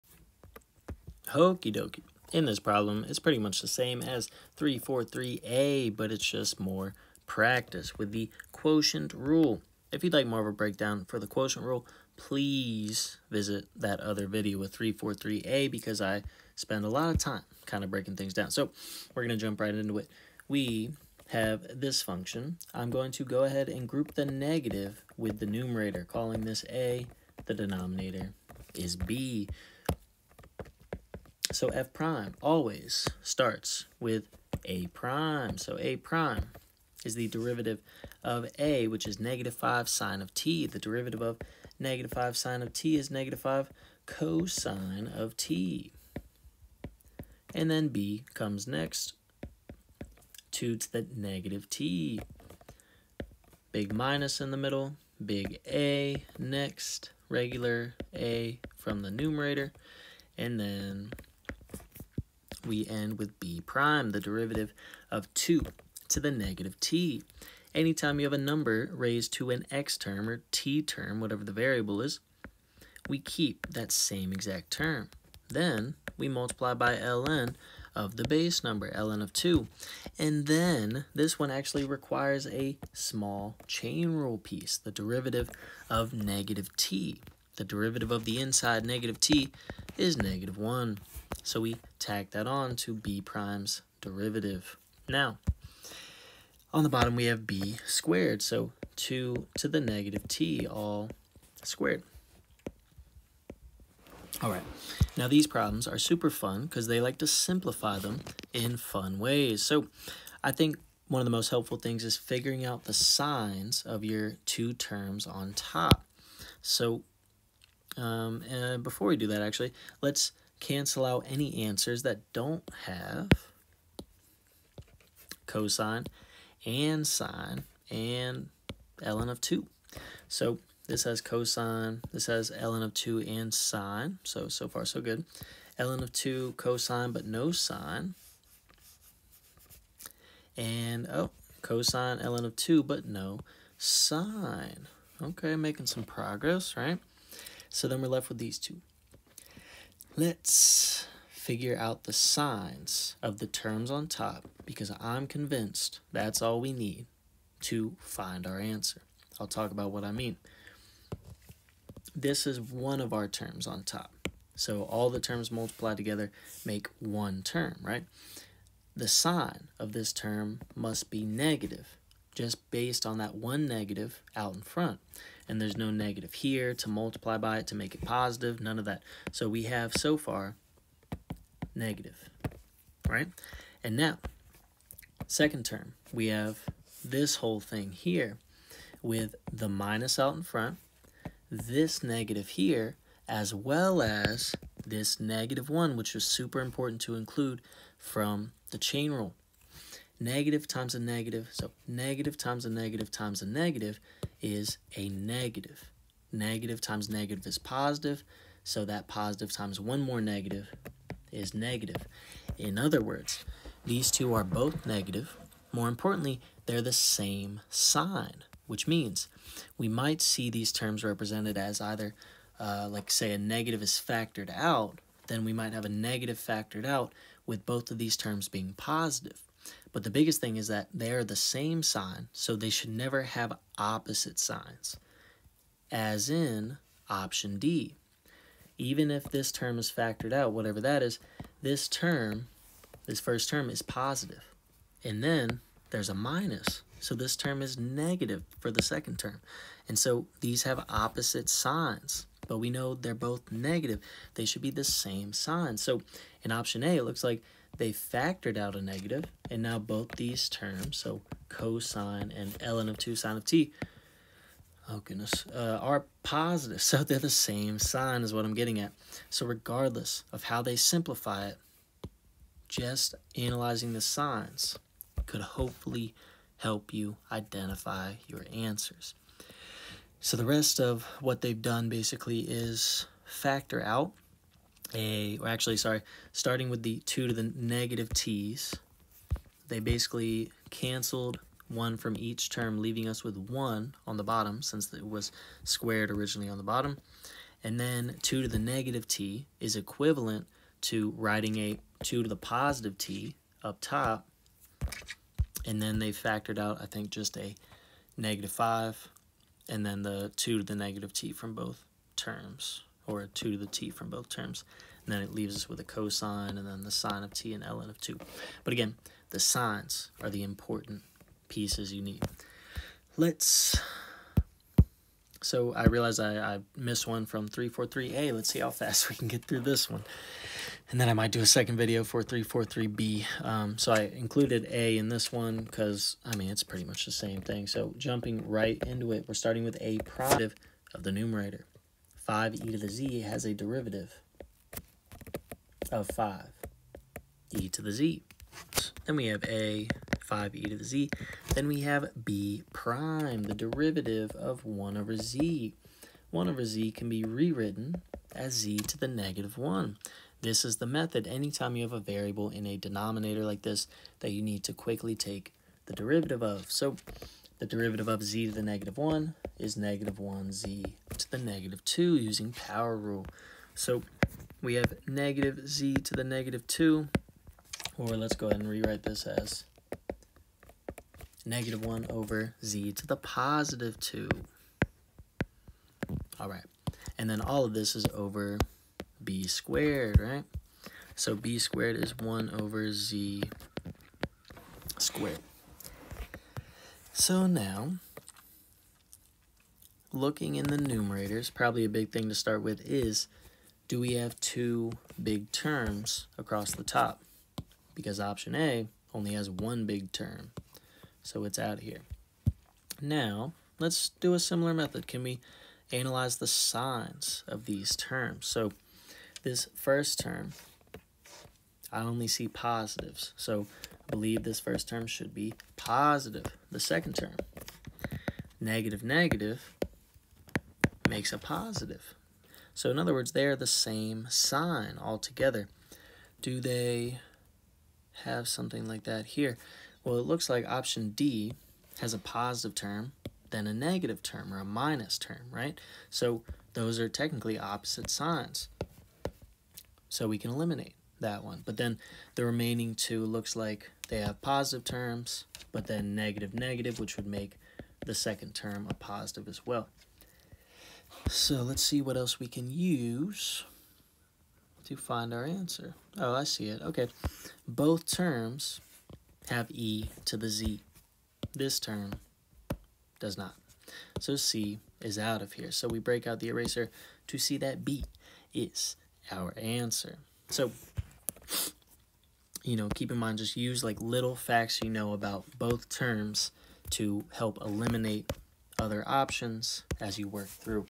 jk these quotient rule problems will blow your mind!!
00:00 Example 1
00:16 Find derivative of the function using Quotient Rule
02:00 Use Process of Elimination to help identify answer
05:25 Example 2